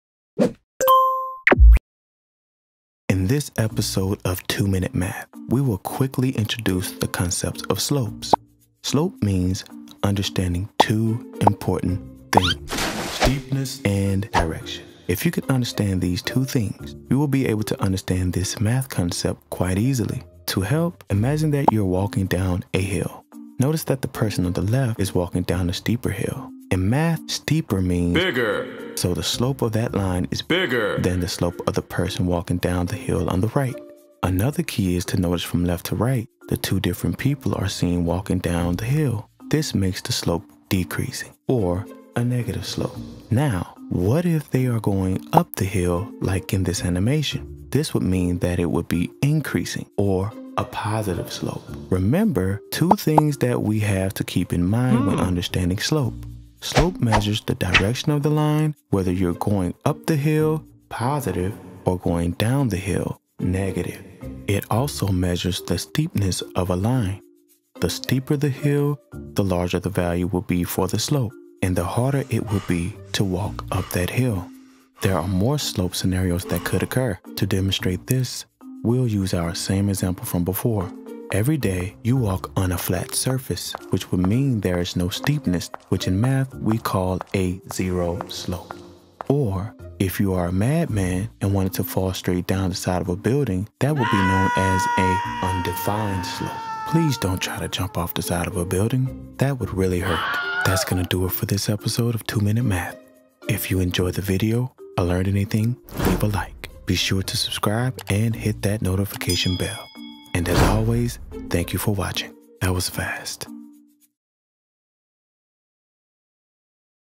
SLOPES Explained in 2 Minutes.
In 2 MINUTES I will explain two important things to remember while working with slopes.  Steepness and Direction. If you can grasp these two things, working with slopes will be a lot easier for you.